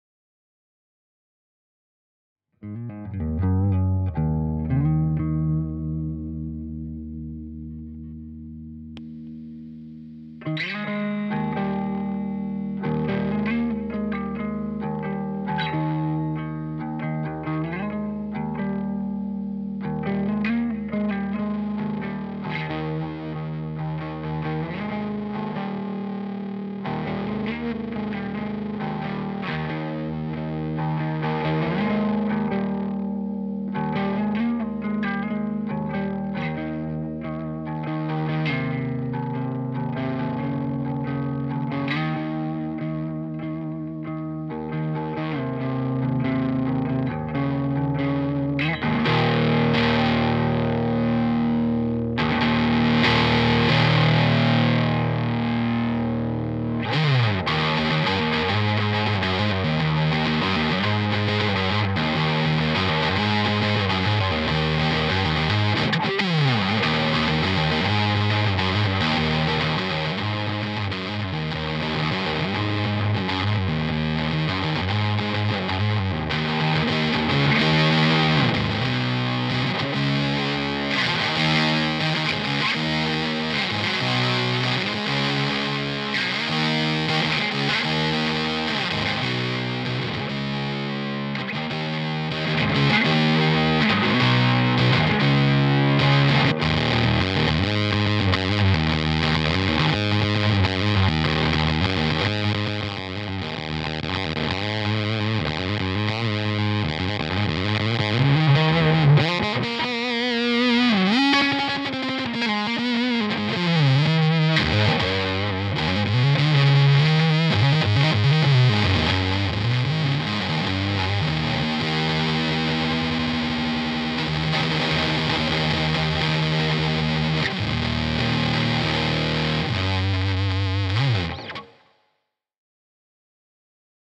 Agent 13 SolidGoldFX
SolidGoldFX Agent 13 - Fuzz pedal review
Recorded with Wild Custom guitar (Suhr pickups SSV/SSH+), FX Amplification Dr B head, Strymon echo, Torpedo Reload.